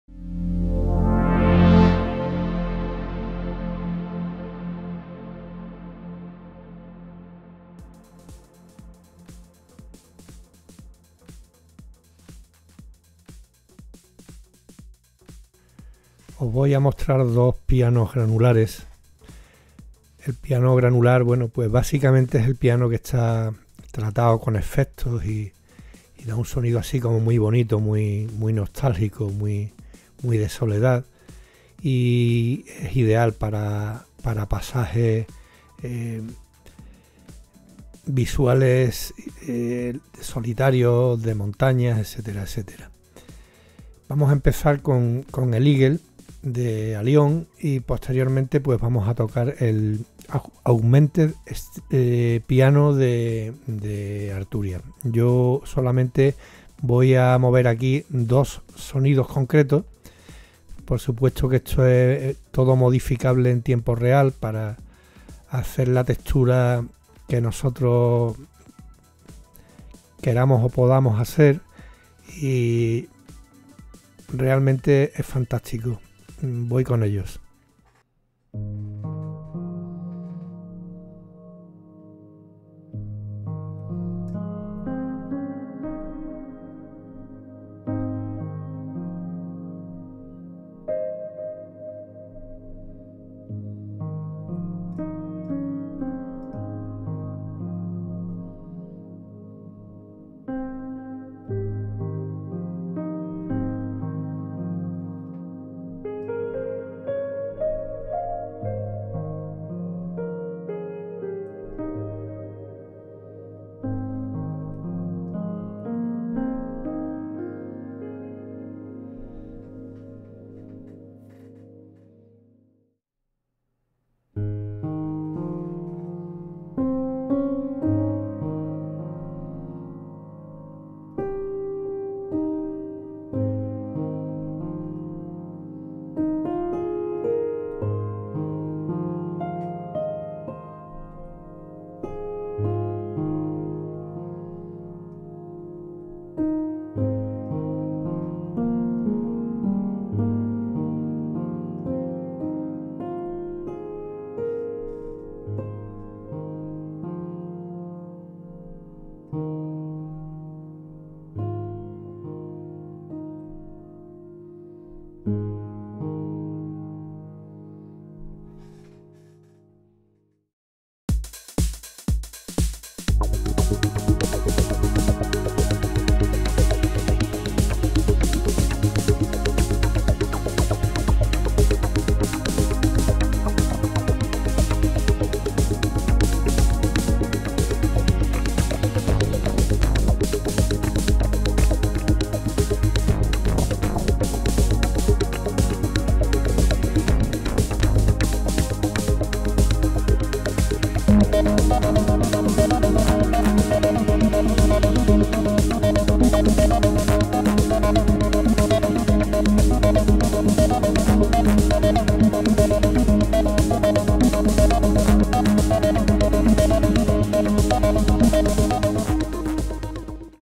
Instrumentos VST Piano granular
Testeo de un par de plataformas que incorporan sonidos de piano granular.